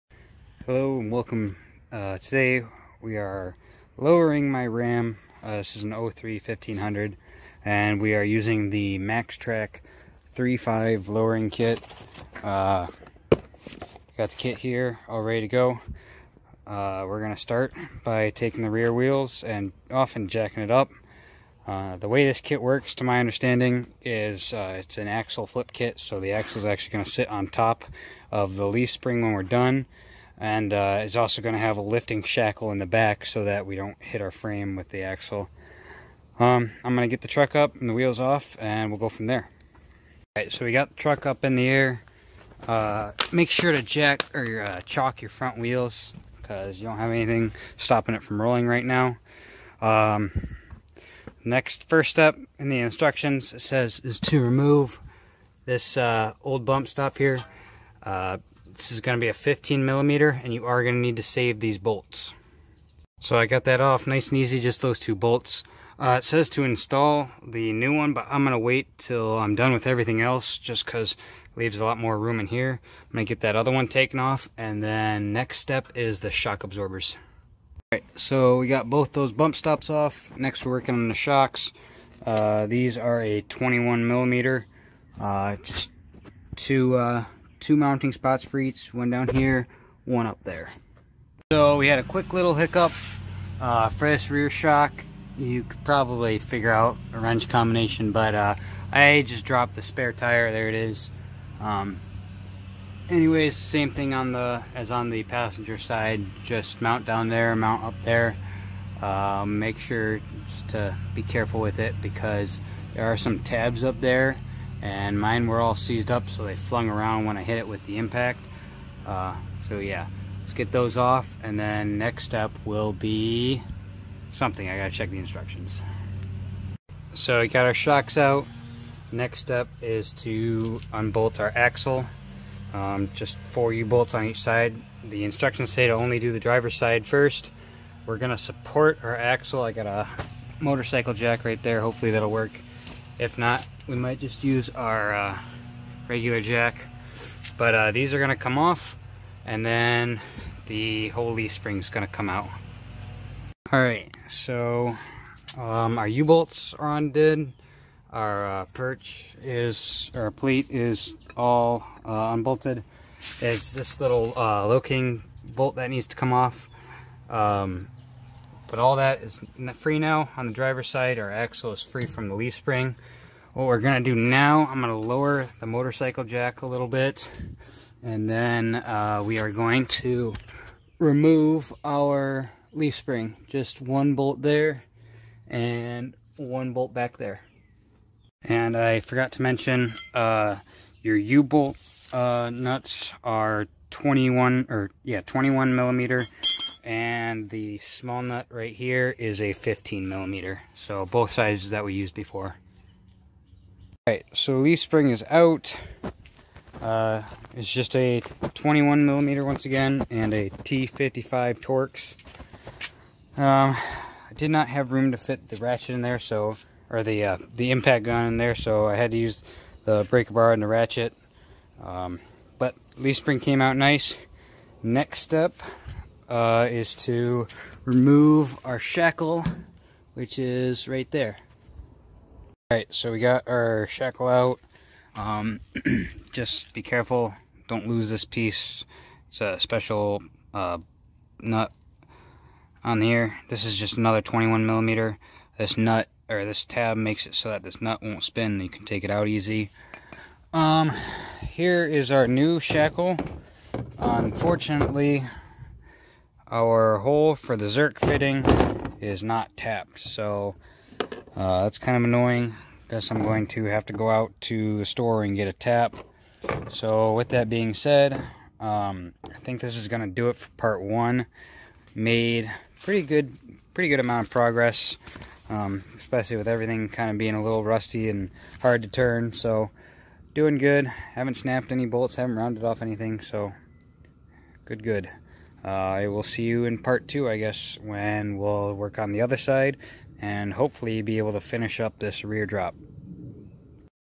Maxtrac 4-5" Rear Flip Kit Install On a 3rd Gen Ram pt. 1
This is pt. 1 of installing the Maxtrac 4-5" rear drop kit on my 2003 Ram